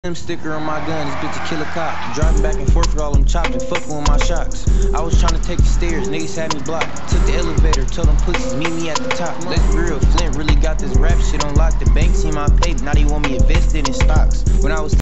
First day out!!!!
Got out of coms and had to appreciate my car